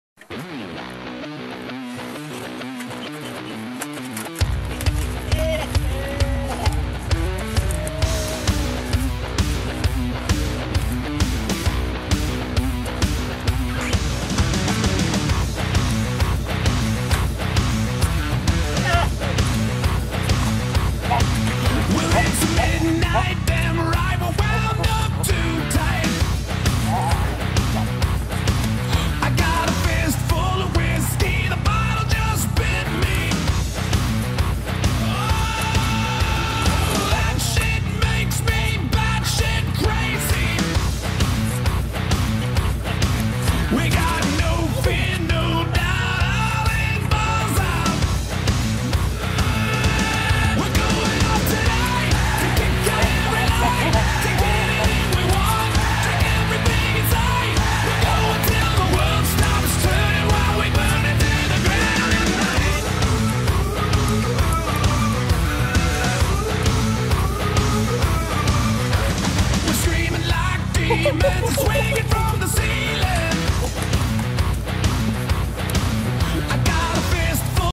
No Laughing Matter & Jeff Farnsworth up to No Good
No Laughing Matter and Firefighter Jeff Farnsworth having some fun before War Indoors 6